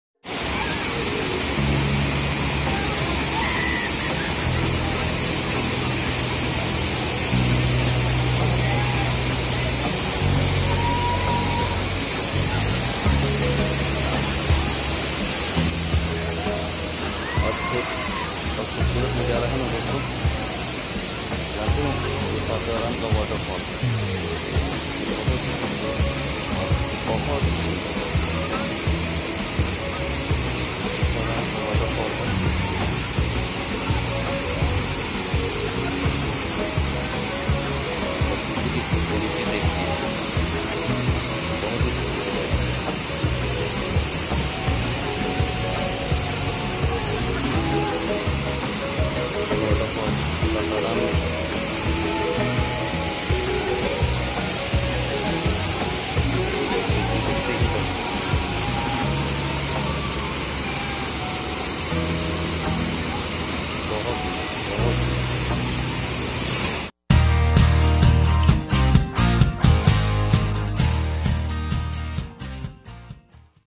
ऐसा अनोखा झरना नहीं देखा होगा| Shasharam's nighra waterfall
Hey friends, is video mein aap Shasharam's ki khubsurat waterfall dekhenge.
Doston ye waterfall Bihar ke Shasharam mein hai, jo behad hi manmohak Or khubsurat vaadiyon se ghirahua hai.
I hope aapko video pasand aayega.
thanks.